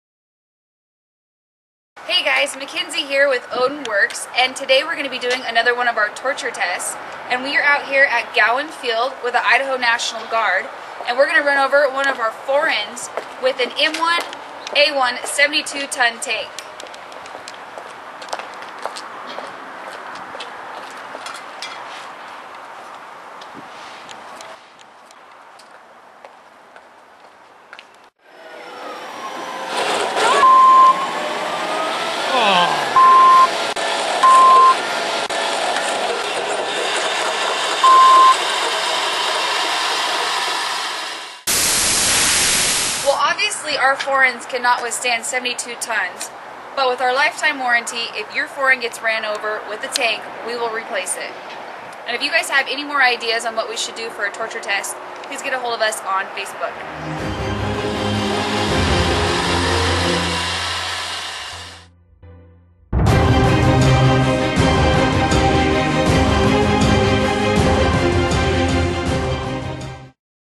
ODIN Works Forend Vs. Tank
ODIN Works forend goes head to head with a M1A1 tank.